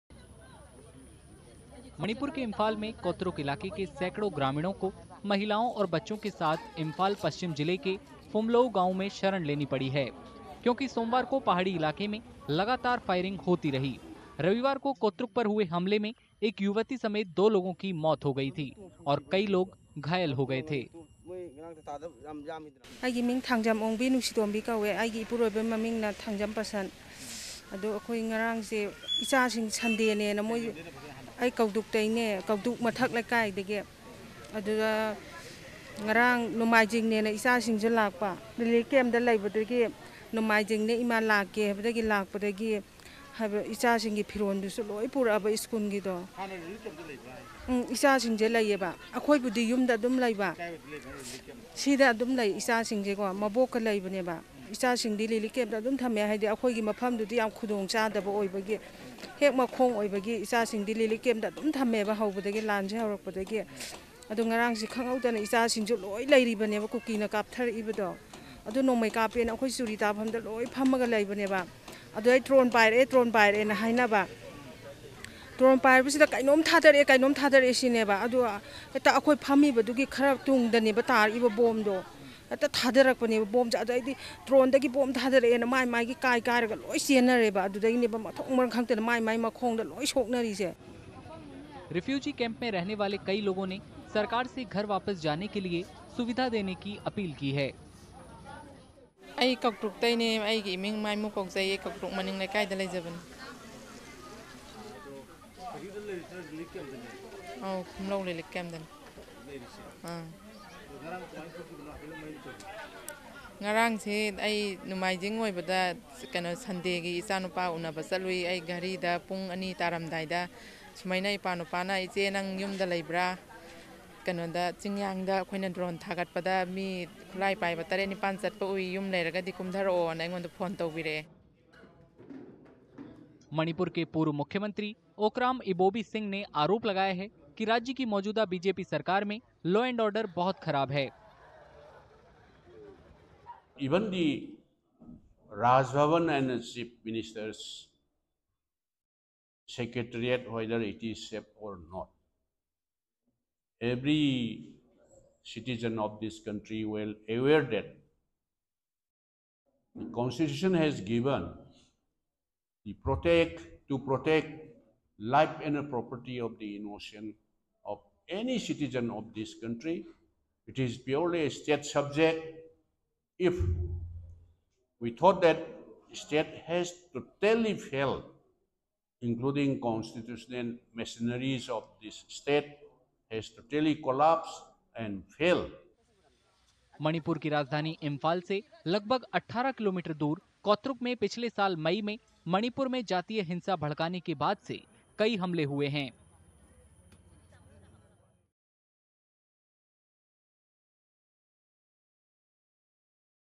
Manipur Firing | मणिपुर के इंफाल में फायरिंग से परेशान लोगों का पलायन | Manipur News
ManipurViolence: मणिपुर के इंफाल में कोत्रुक इलाके के सैकड़ों ग्रामीणों को महिलाओं और बच्चों के साथ इंफाल पश्चिम जिले के फुमलोऊ गांव में शरण लेनी पड़ी है, क्योंकि सोमवार को पहाड़ी इलाके में लगातार फायरिंग होती रही। रविवार को कोत्रुक पर हुए हमले में एक युवती समेत दो लोगों की मौत हो गई थी और कई लोग घायल हो गए थे